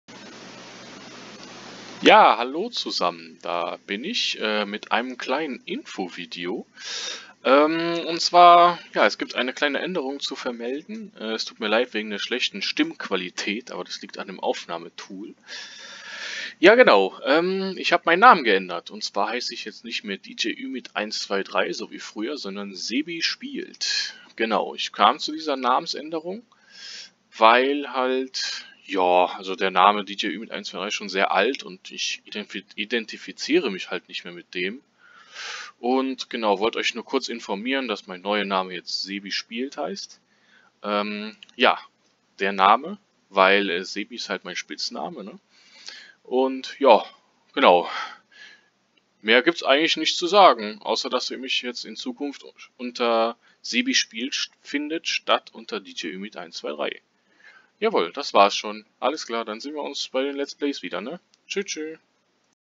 Infovideo - Namensänderung in SebiSpielt
Hallo zusammen,
hiermit wollte ich euch nur kurz mitteilen, dass ich meinen Namen in SebiSpielt ändere und ihr mich in Zukunft unter diesem Namen findet.